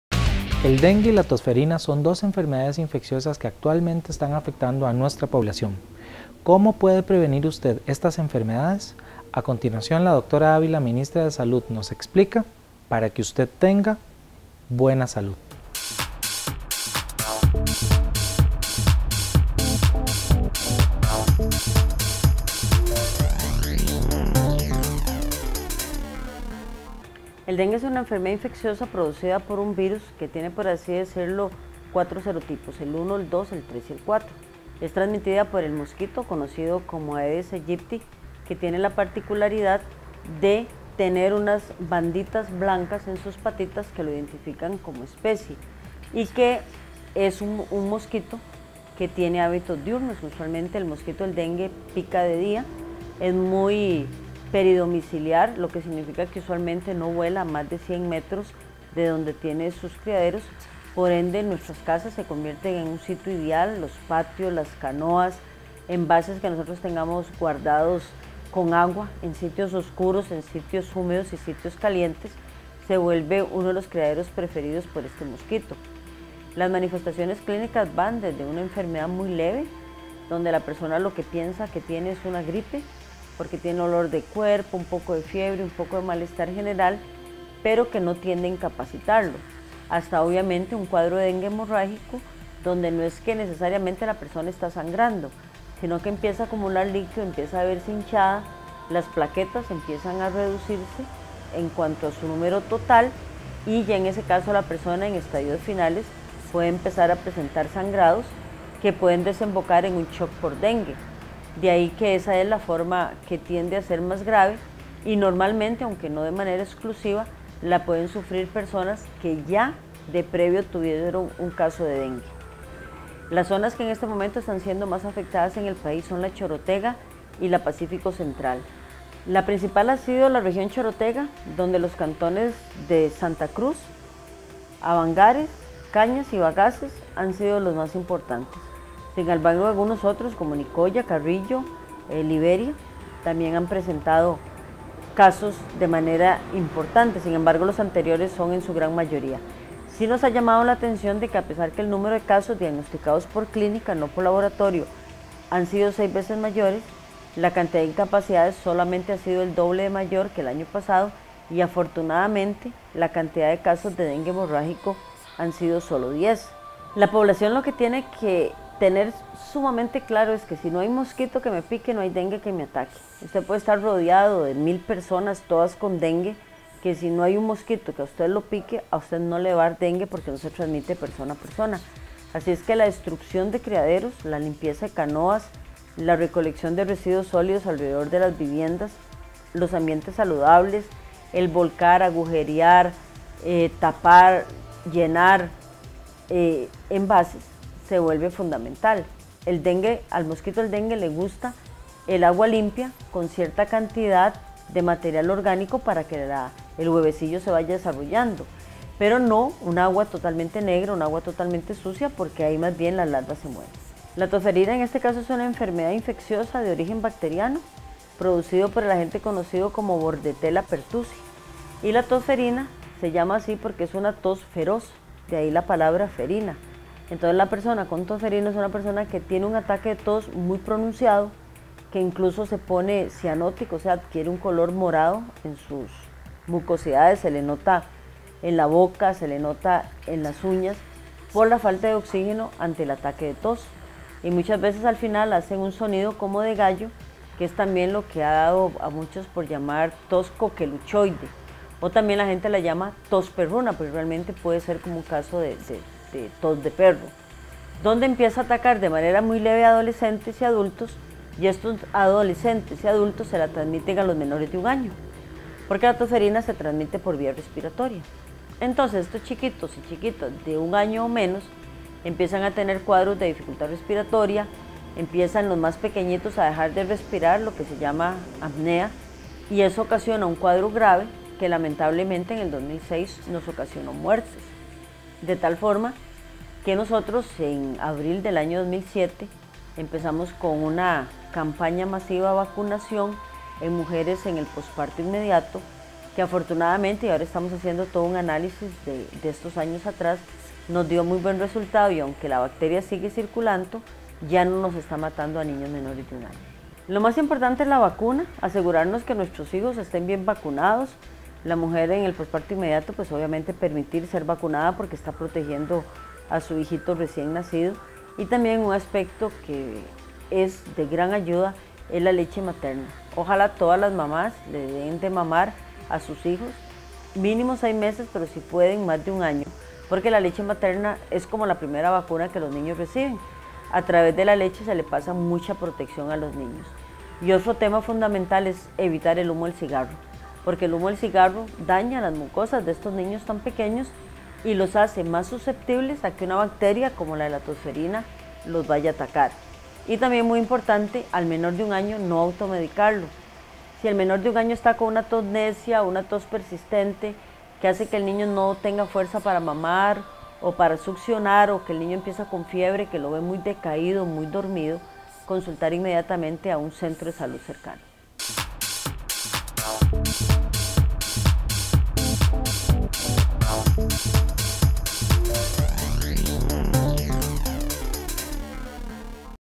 Dengue y tosferina en Costa Rica
El dengue y las tosferina son dos enfermedades infecciosas que prácticamente habían desaparecido en Costa Rica. Sin embargo, en años recientes aparecen de nuevo en el panorama de salud el país. La Dra. Maria Luisa Ávila, Ministra de Salud de Costa Rica, nos explica cómo podemos prevenir estas enfermedades.